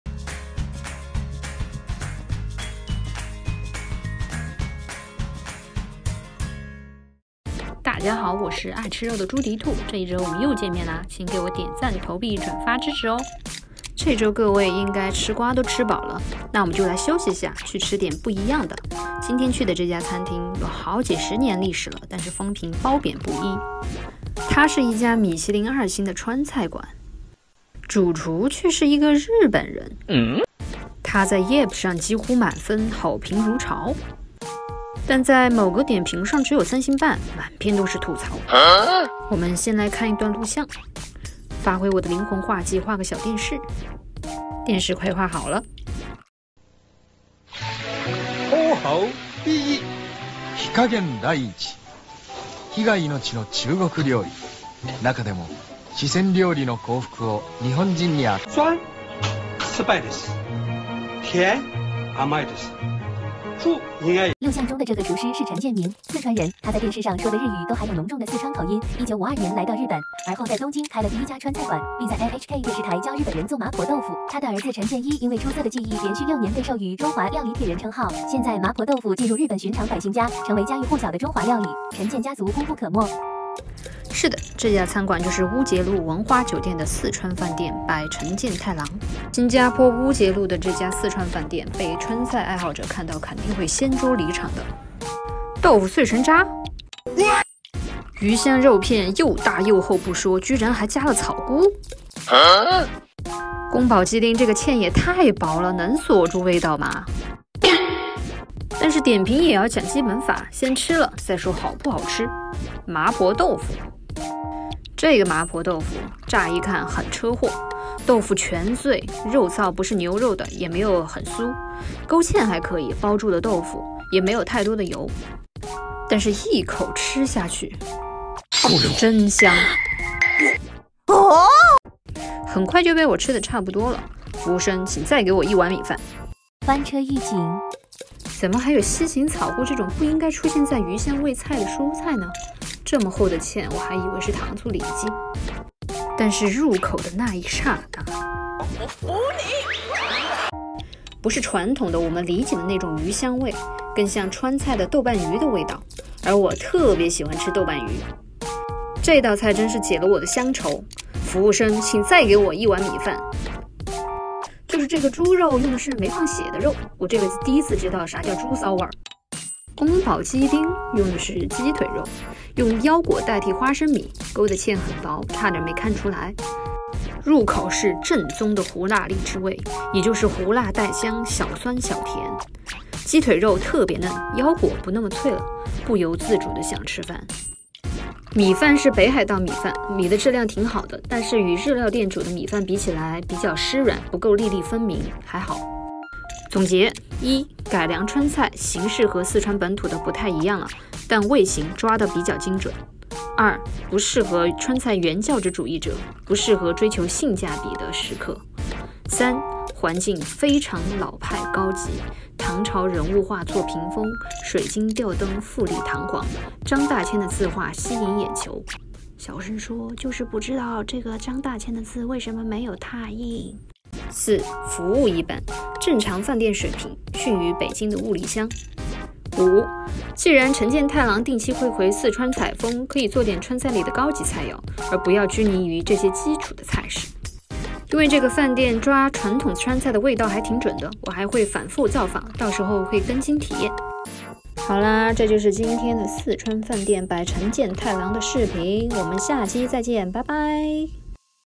Going Around, Eating Sichuanese Food| Singpaore Michelin 2 star Restaurant: Shisen Hanten
Sichuanese food.